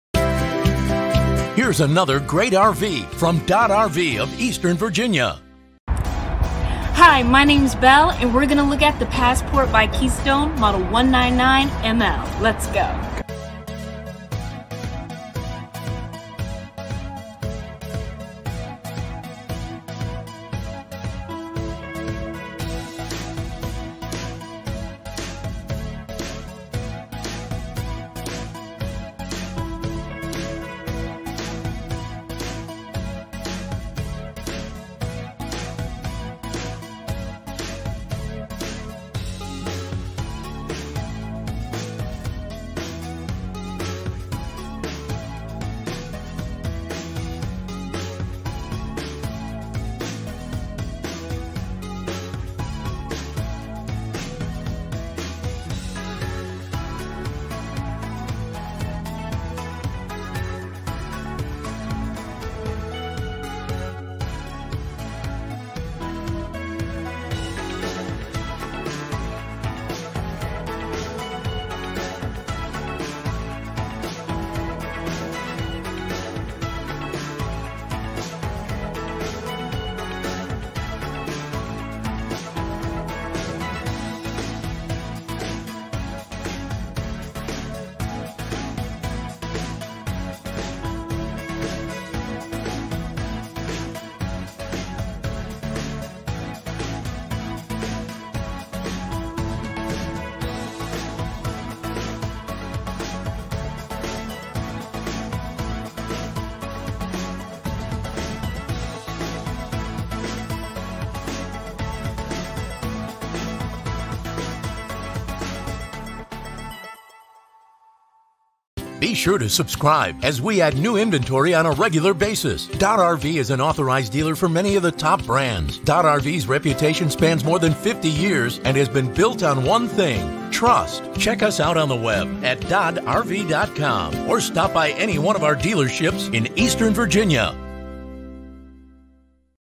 Keystone-Passport SL Series-199ML - by Dodd RV of Portsmouth and Yorktown, Virginia
Keystone Passport travel trailer 199ML SL Series highlights:
Murphy Bed
Exterior Speakers
U-Shaped Dinette
Single Slide
Bathroom Wardrobe

Envision relaxing on the sofa while the family plays a board game at the U-shaped dinette slide in this Passport travel trailer. You can enjoy the front living area during the daytime, and at night this space can transform into a sleeping area once the Murphy bed has been folded down. The full rear bathroom allows you to skip the public facilities, and it also has a large wardrobe so that you can store items like your towels and toiletries. The full kitchen provides the necessary appliances to make your favorite meals, like a three-burner range, a gas oven, and a microwave, and you will also find plenty of storage for all your things with interior storage, as well as in the exterior pass-through storage compartment.

The Passport SL Series travel trailers by Keystone RV come in a compact design with travel-friendly lengths that are lightweight and easy to tow behind many smaller crossover SUVs. You will find several residential-style features such as durable Armstrong vinyl flooring, residential interior doors, and upgraded leather furniture. Plus, there is also a stainless steel sink with a pull-down faucet. The exterior shower is very useful as you get cleaned up before entering the RV, wash your pets, or even rinse a few dishes outside. Choose your favorite Passport SL Series today, and start going everywhere!

Yorktown, VA
7023 George Washington Memorial HWY
Yorktown, VA 23692

Portsmouth, VA
4705 Portsmouth Blvd
Portsmouth, VA 23701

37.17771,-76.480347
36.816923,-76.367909